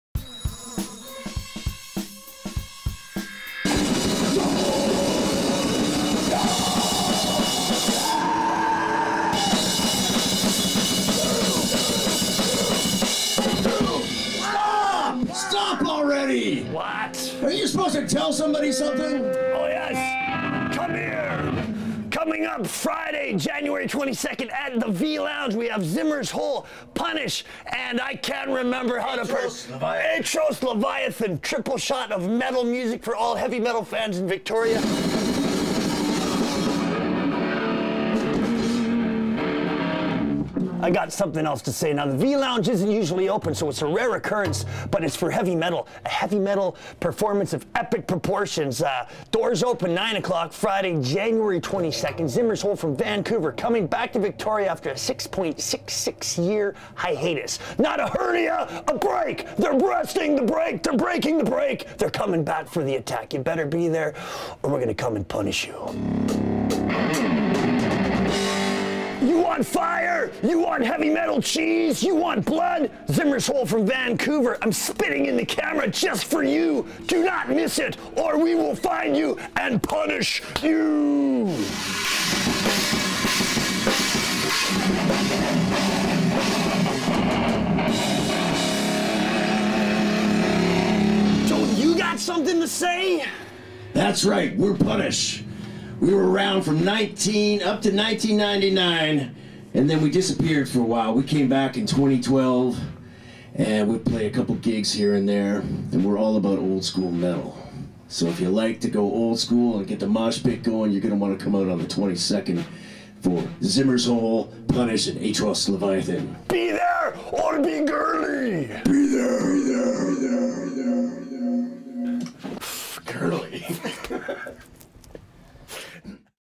Zimmer's Hole, Punish & Atrous Leviathan@ V-Lounge Shaw TV Victoria
Victoria metal band "Punish" give us the goods on a reunion, one-off concert with Vancouver band Zimmer's hole, along with Atrous Leviathan at the V-Lounge this Friday, Jan 22nd.  Share this among the group!